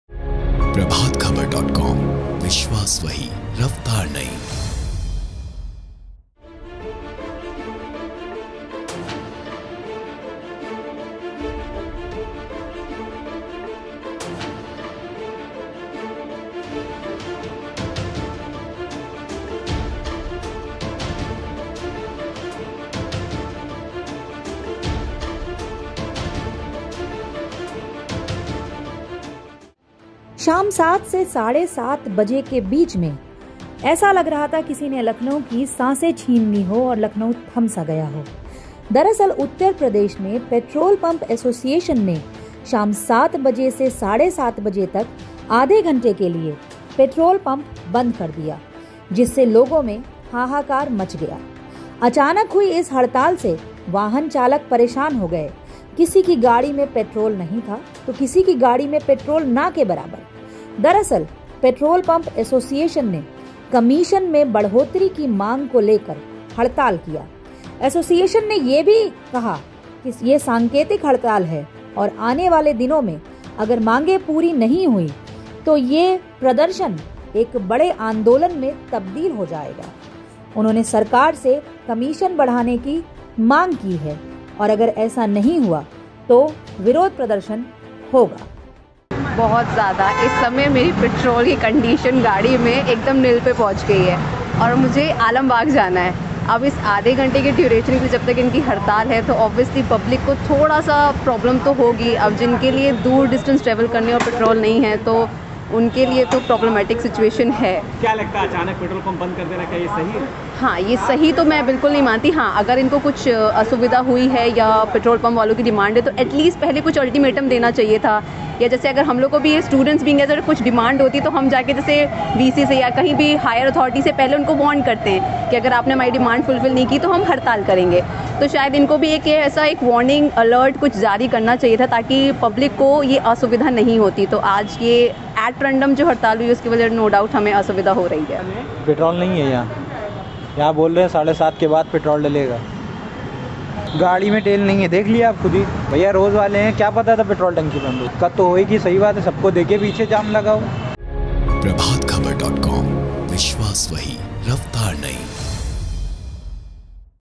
Lucknow News: डीलर्स की कमीशन बढ़ाने की मांग, आधे घंटे के लिए पेट्रोल पंप बंद, आम जनता रही परेशान
उत्तर प्रदेश में पेट्रोल पंप एसोसिएशन ने शाम सात से साढ़े सात बजे तक आधे घंटे के लिए पेट्रोल पंप बंद कर दिया. जिससे लोगों में हाहाकार मच गया आधे घंटे के लिए पूरा लखनऊ थम गया,अचानक हुई इस हड़ताल से वाहन चालक परेशान हो गए किसी की गाड़ी में पेट्रोल नहीं था तो किसी की गाड़ी में पेट्रोल ना के बराबर,
दरअसल पेट्रोल पंप एसोसिएशन ने कमीशन में बढ़ोत्तरी की मांग को लेकर हड़ताल किया था. एसोसिएशन ने कहा है कि यह सांकेतिक हड़ताल है और आने वाले दिनों में बड़े पैमाने पर प्रदर्शन होगा. उन्होंने सरकार से कमीशन बढ़ाने की मांग की है. ऐसा नहीं होने पर विरोध-प्रदर्शन की बात कही गई है.